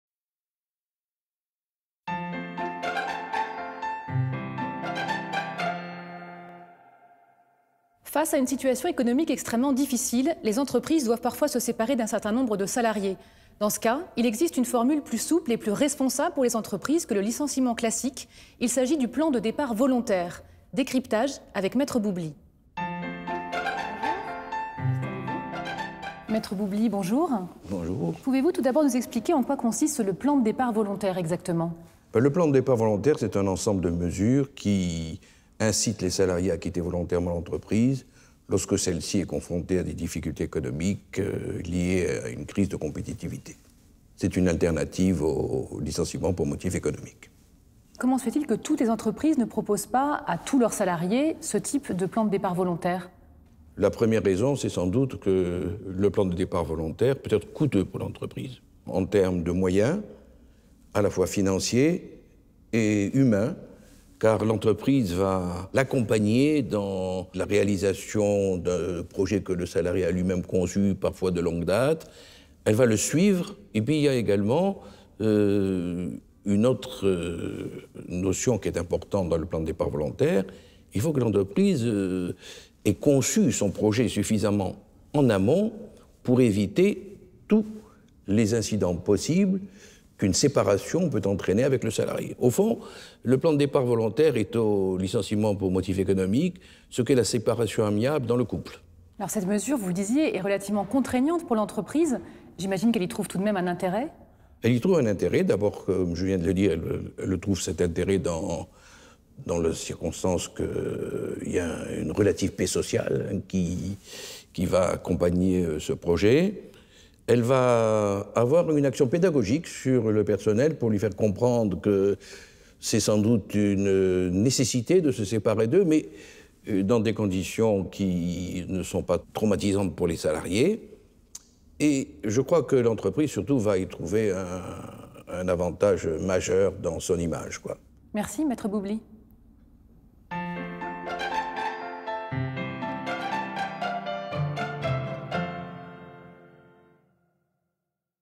Le plan de départ volontaire - Bernard Boubli - Capstan TV
Bernard Boubli, Avocat Associé, répond à vos questions sur le plan de départ volontaire (PDV).